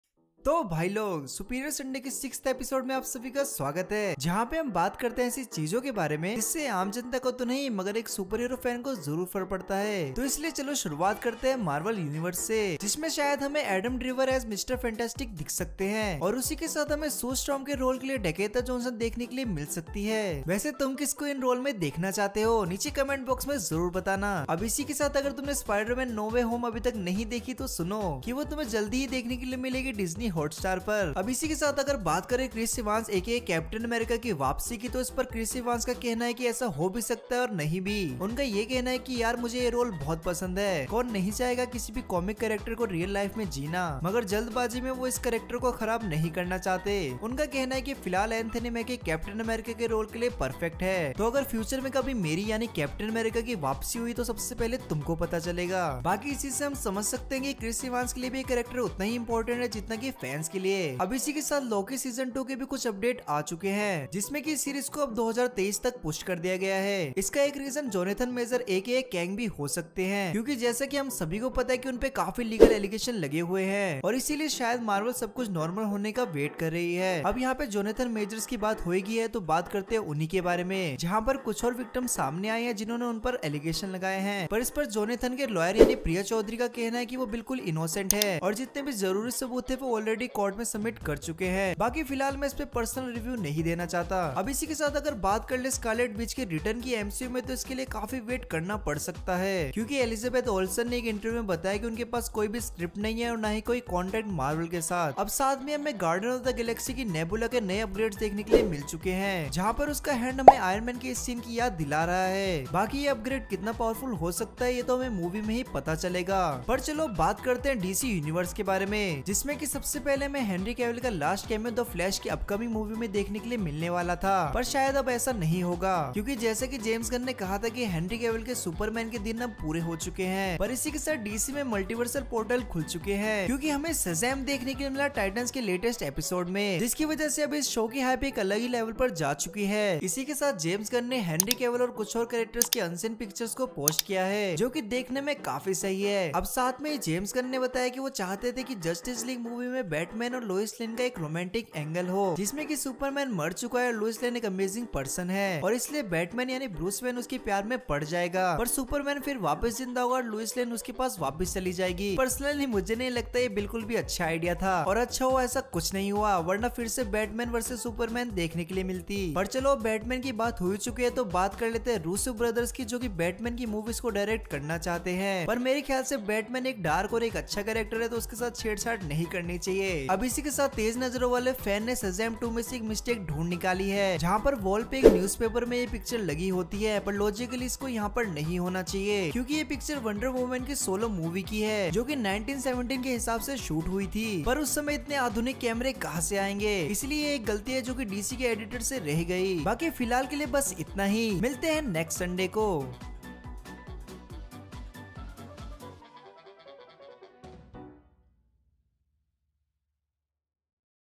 Adam driver joins Marvel ? , Chris Evans is back , Jonathan majors case updates | SUPERIOR LADKA
Hi  guys Superior ladka here
Hope you enjoy 6th Episode of Superior SundaY
She Hulk all powers explained [  Ye Kon Kon si powers hai jo Hulk ke pass bhi nahi ]